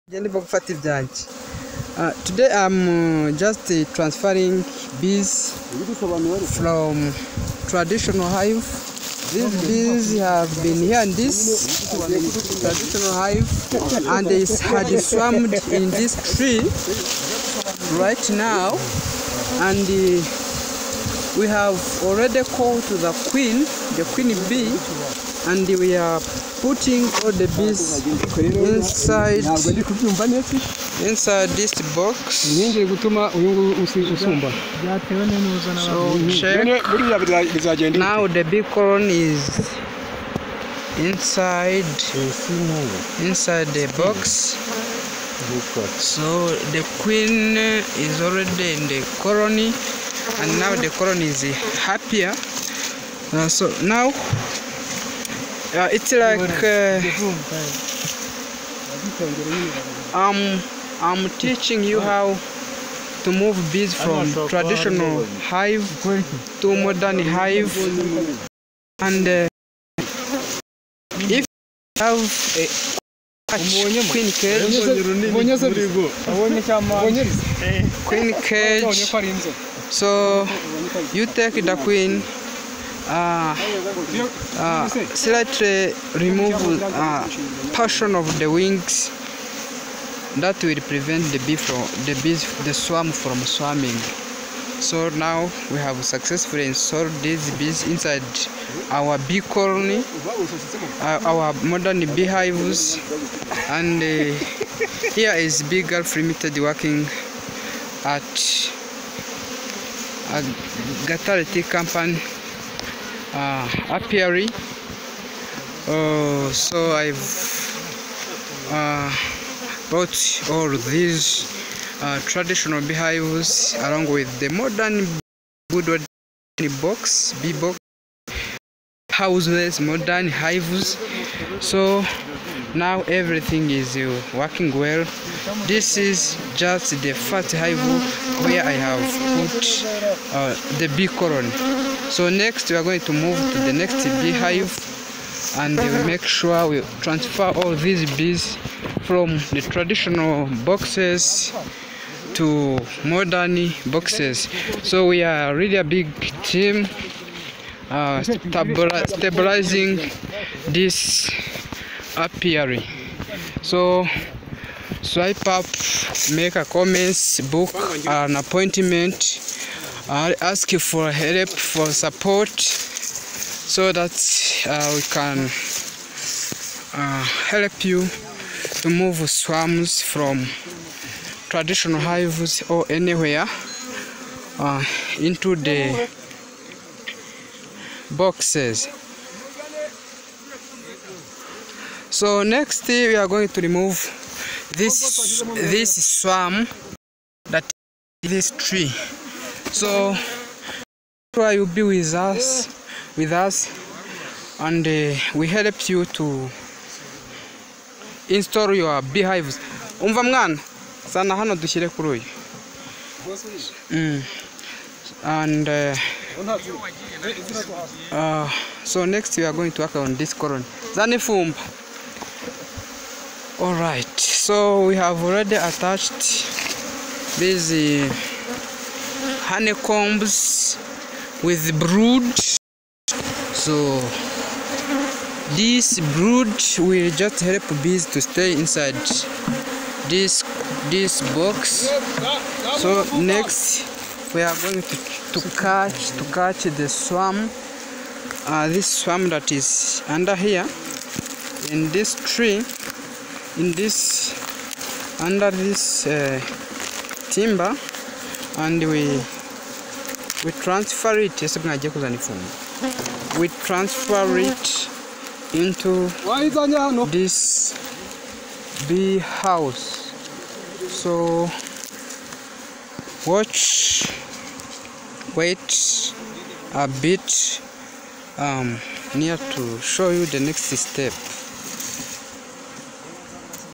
How to move bees from traditional hive to modern bee hive
This video describes what you can do to move bees  when you have swarm With no bee blood